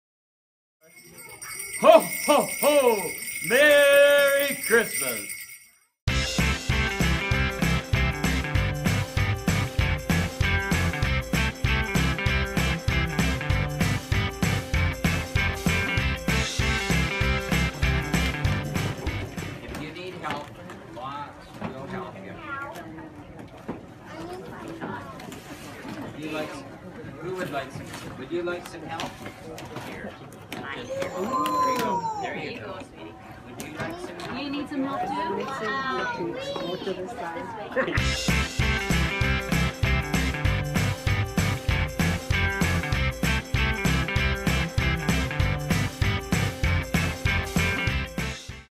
TLC@Home 2015
Cameco, Global TV, Hairstyle Inn and 96.3 CRUZ FM are partners again for TLC@Home this holiday season. Watch the reaction as children at a Saskatoon core community school open their shoebox Christmas gifts.